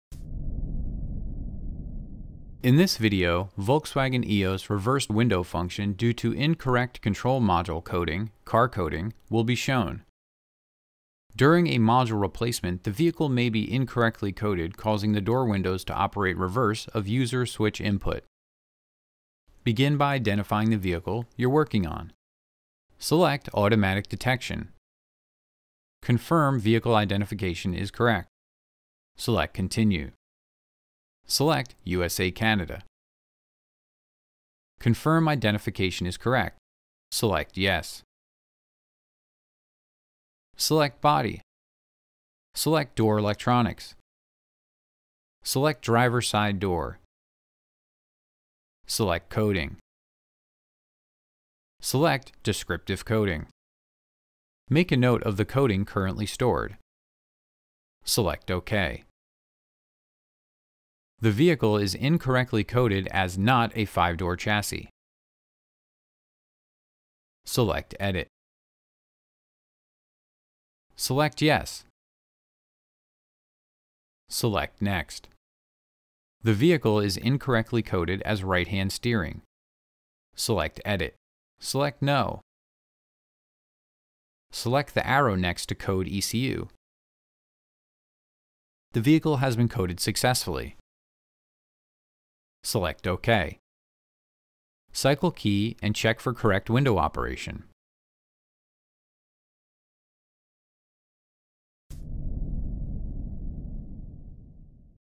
VW Eos Door Module Reverse Coding
In this video VW Eos reversed window function due to incorrect control module coding (car coding) will be shown. During a module replacement the vehicle may be incorrectly coded, causing the door windows to operate reverse of user switch input.